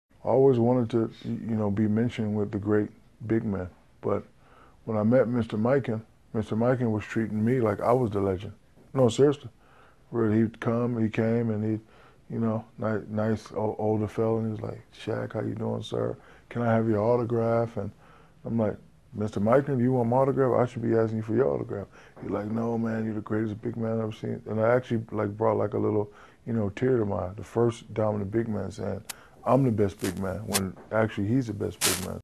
Shaquille O'Neal sound on meeting George Mikan and Mikan asking for his autograph (2009)
Shaq talks about meeting Mr. Mikan and what it meant for George Mikan to ask for his autograph.